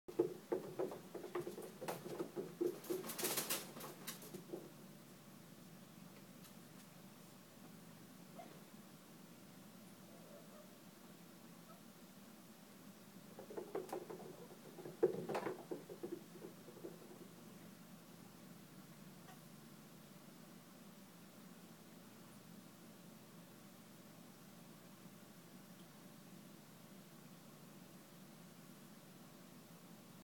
Maltese Munchkin's playmates learn the GoPet treadwheel with NO training!
Maltese Munchkin's pals , two queen Munchkin Napoleon breed cats ( this breed approximately 5 to 6 pound cats with dwarf legs so mine have never jumped on counters) take their turn on the GoPet non-motorized treadwheel for fun exercise, and
this one queen takes to it naturally with no training, no treats and no lasers. Instant raw, untrained, instinctive love of this GoPet is captured in this video!